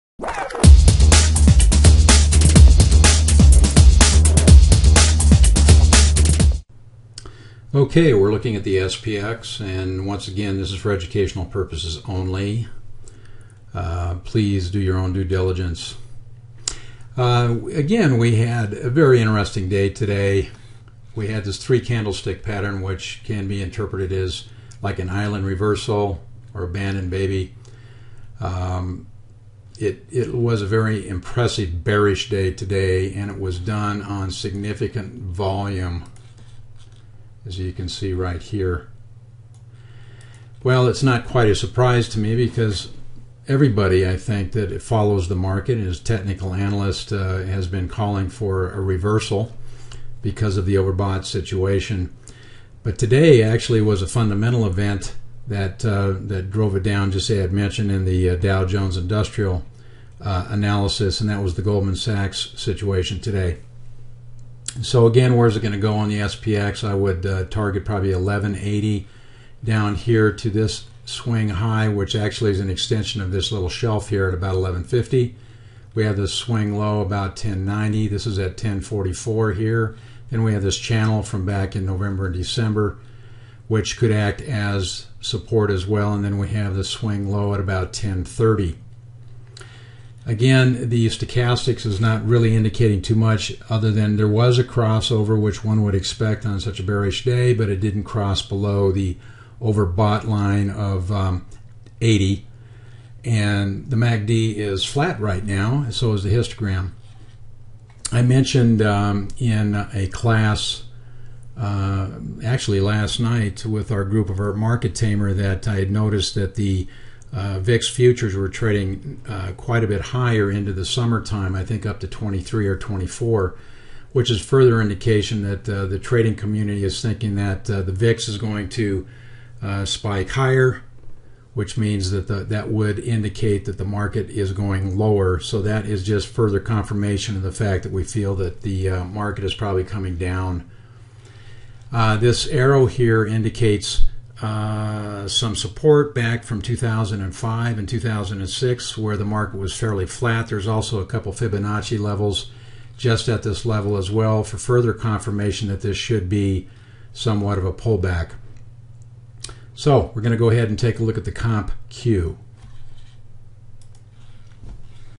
Is The Goldman News The Catalyst For The Next Big Sell Off? S&P 500 4-16-10
The market pulled back today on big volume on news of alleged fraud by Goldman Sachs. Goldman reports on Tuesday with Citi on Monday. I am looking for some follow through next week as a result of today's events.